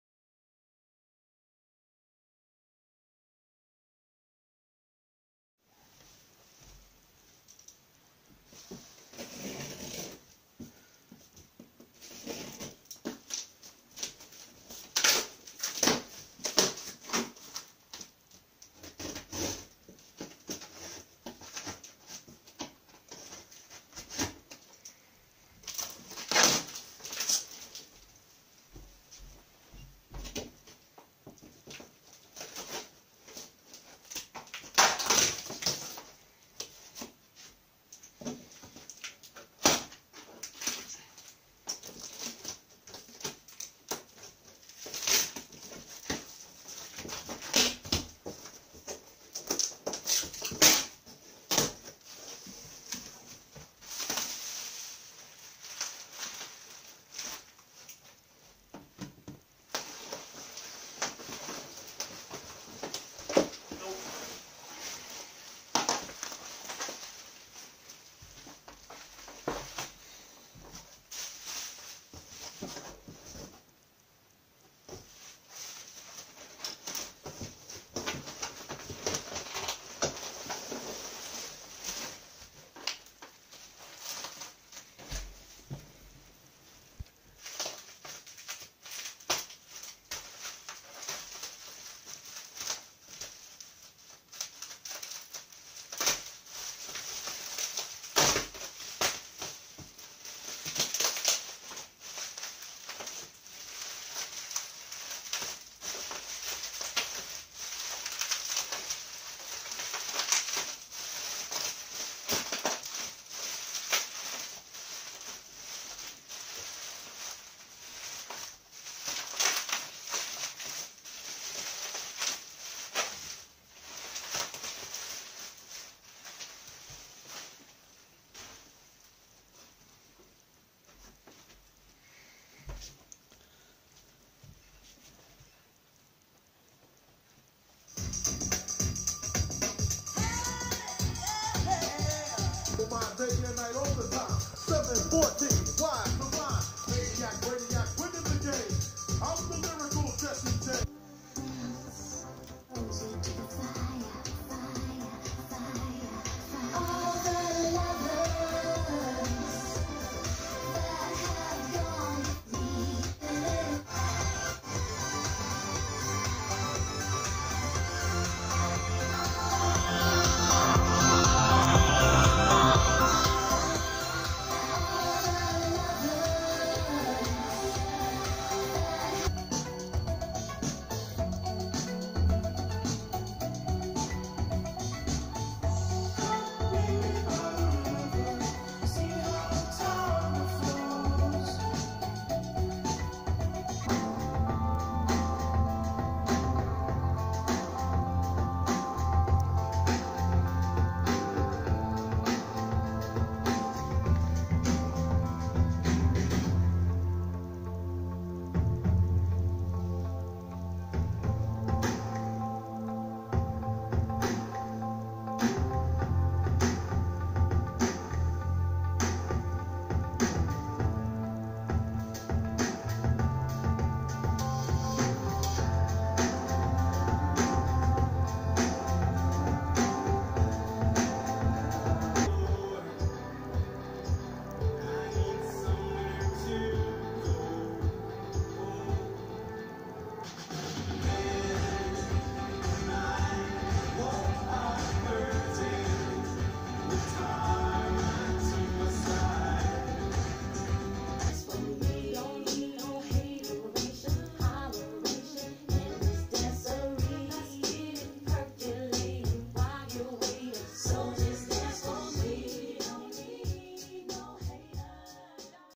JBL CONTROL 5 MONITOR SPEAKERS UNBOXING AND TEST!
Excellent pair of JBL Control 5 speakers, in truly amazing condition with intact foam surrounds, all of which are a huge rarity considering their age! These are some of the best JBL speakers you can get for the tone of their sound, it has the JBL sound signature written all over it. If you ever see a pair, GET a pair without even considering it.